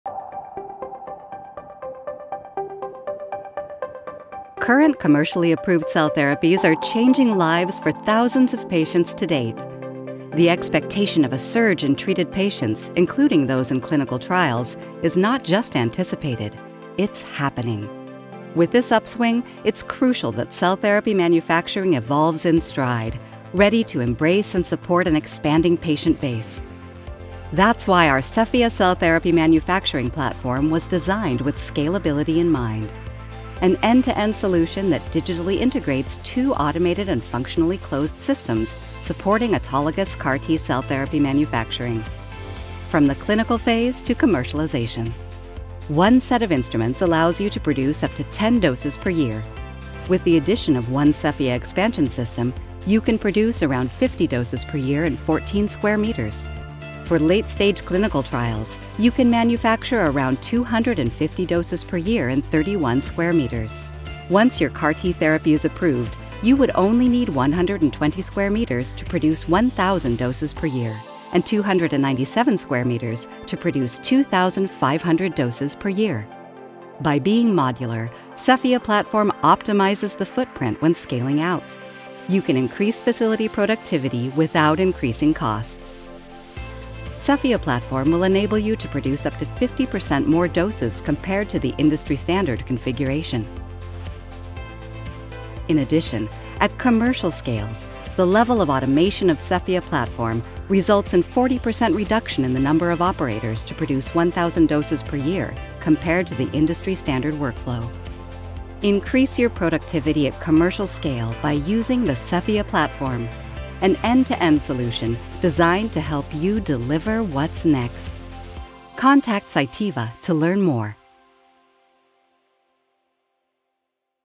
How the Sefia™ platform boosts cell therapy manufacturing
The Sefia™ cell therapy manufacturing platform is transforming the way cell therapies are produced. Designed for scalability, it provides an end-to-end solution for the cell and gene therapy manufacturing process. This innovative platform integrates automation and digital connectivity, streamlining the cell therapy process from clinical development to commercialization.

Key features of the Sefia platform:
• A modular design for maximum flexibility in cell therapy workflows.
• Automated, functionally closed systems that enhance efficiency and reduce contamination risk.
• Scalability to support a growing number of doses as manufacturing needs expand.

The Sefia platform simplifies the cell therapy manufacturing process, improving productivity and consistency at every stage. By digitally integrating with other Cytiva solutions, Sefia systems enable a seamless workflow from the clinical phase to full commercialization, helping manufacturers to deliver cutting-edge therapies to patients faster and more reliably.

Discover how the Sefia platform empowers the future of cell therapy by delivering scalable, reliable, and efficient manufacturing solutions.

Chapters:
00:05 Relevance of CAR T cell therapy manufacturing
00:30 Meet Sefia™ platform
00:46 Potential doses per square meter
01:16 A modular platform